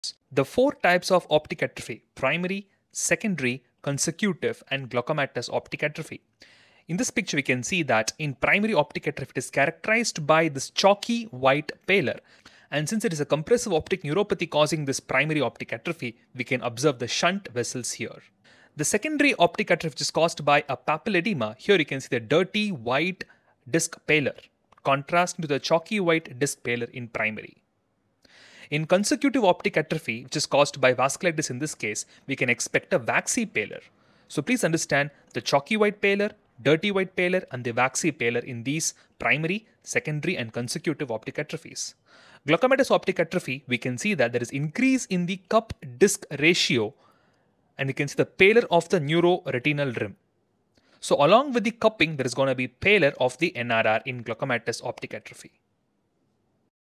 Optic Atrophy - Clinical Appearance of Each Type
How each type of optic atrophy (OA)  look and what are the names given for each type of disc pallor?

- Primary OA = Chalky white
- secondary OA = Dirty white
- Consecutive = Waxy pallor
- Glaucomatous OA = NRR loss/ cupping/ increased cup disc ration + pallor of NRR